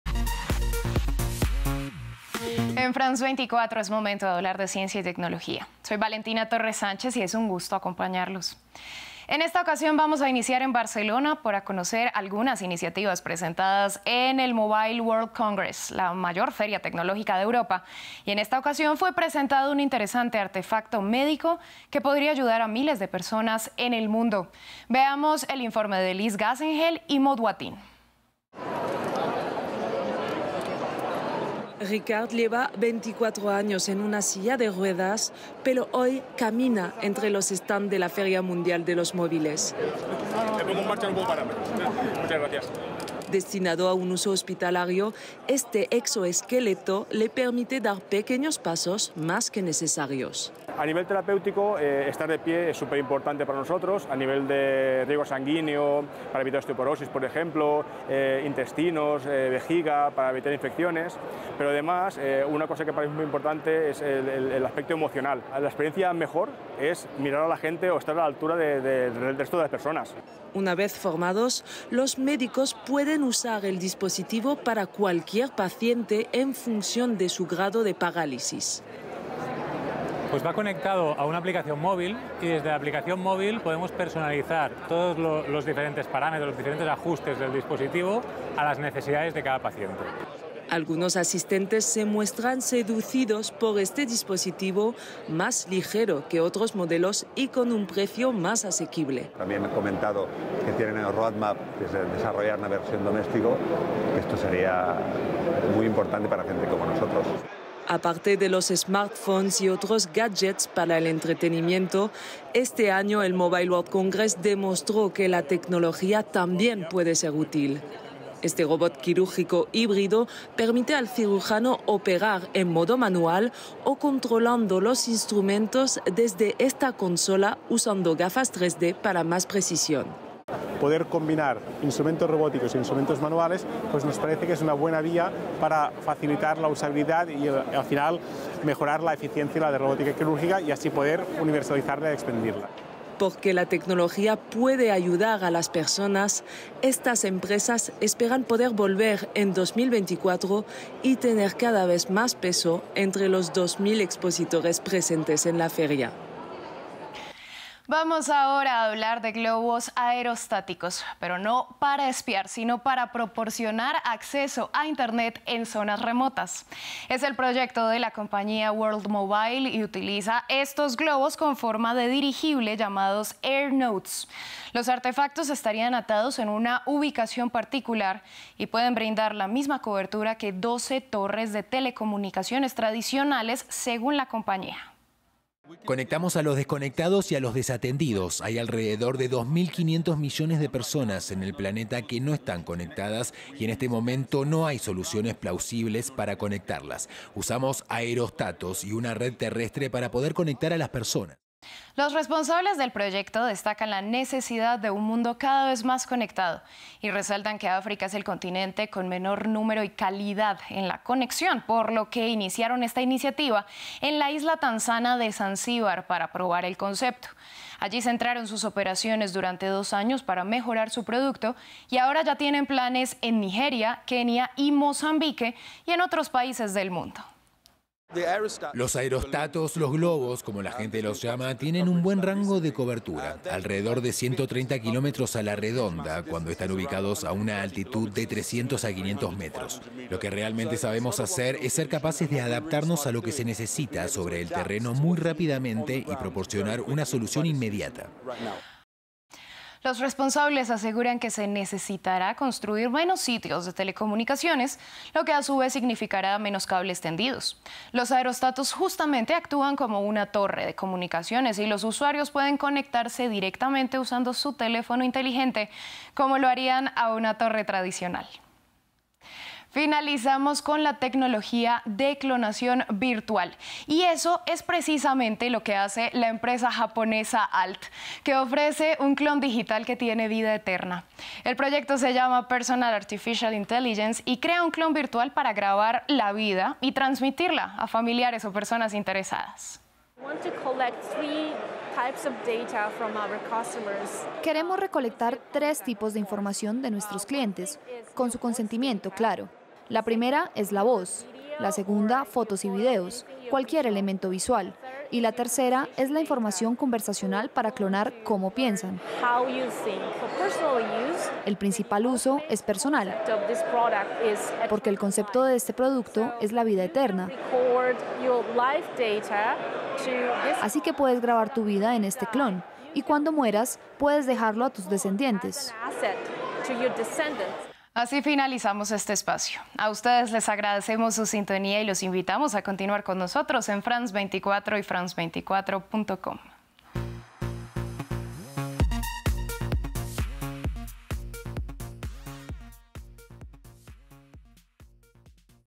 Avances médicos y tecnológicos marcaron el Mobile World Congress de Barcelona
La edición 2023 del Mobile World Congress se llevó a cabo entre el 27 de febrero y el 2 de marzo. Innovaciones médicas y tecnológicas estuvieron bajo la lupa en la que es considerada la feria tecnológica más importante de Europa.